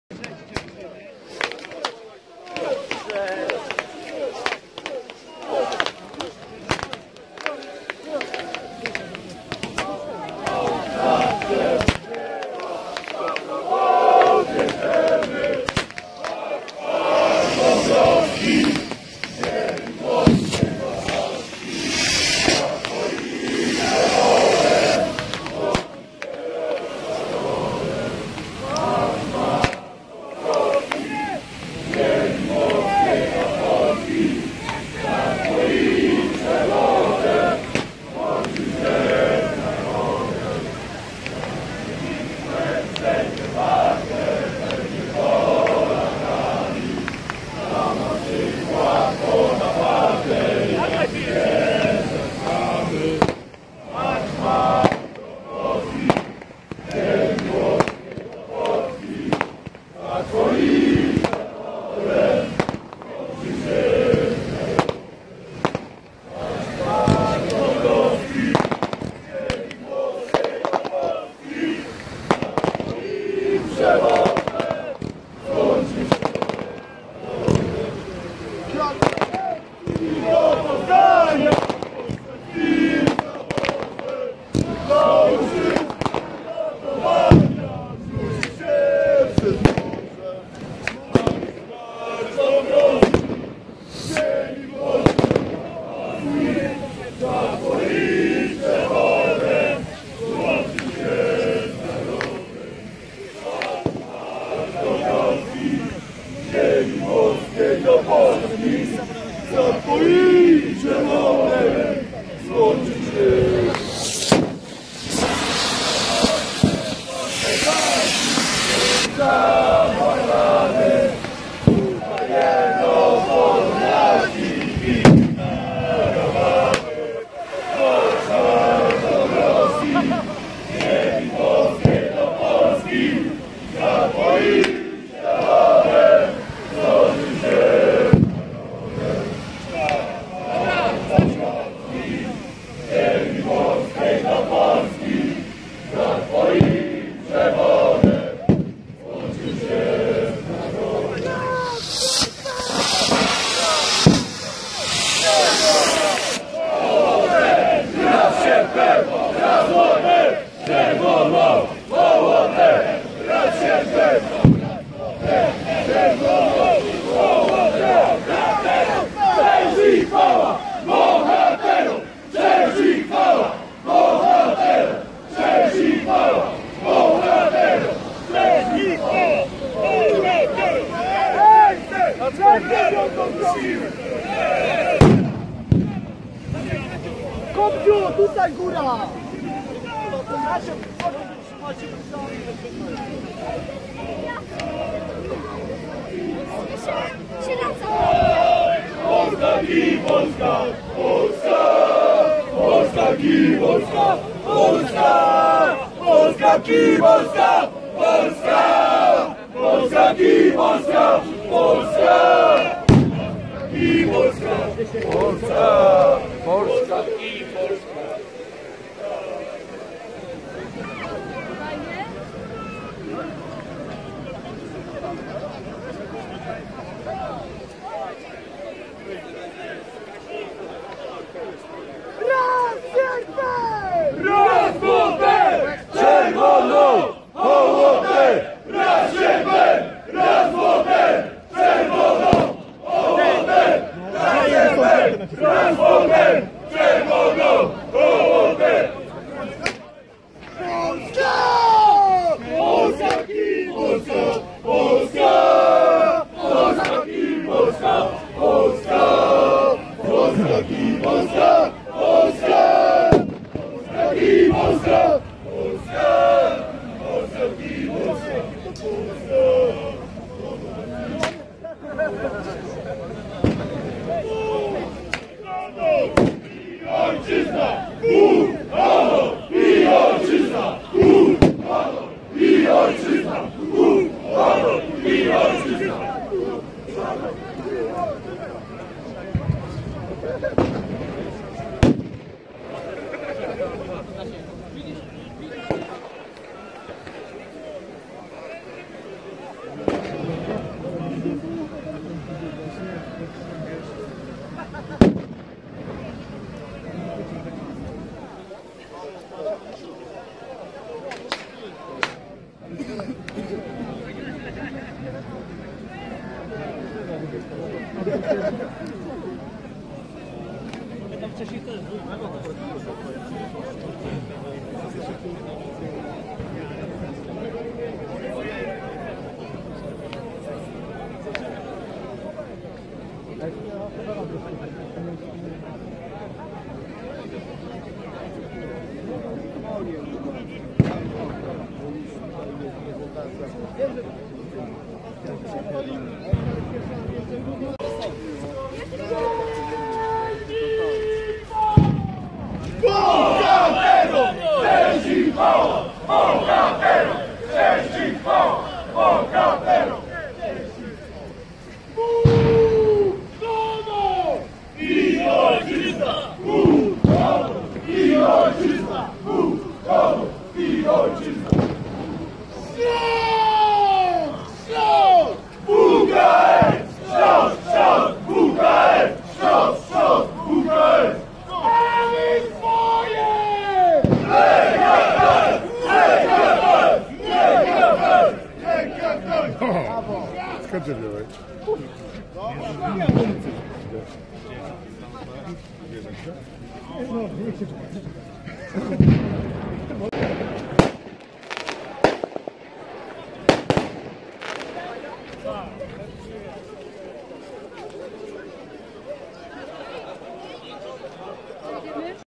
12 PATRIOTYCZNA PIELGRZYMKA KIBICÓW NA JASNĄ GÓRĘ 11.01.2020 ( oprawa kibiców)
12 PATRIOTYCZNA PIELGRZYMKA KIBICÓW NA JASNĄ GÓRĘ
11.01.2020

Ideą przewodnią tegorocznej edycji były obchody 100 rocznicy Bitwy Warszawskiej.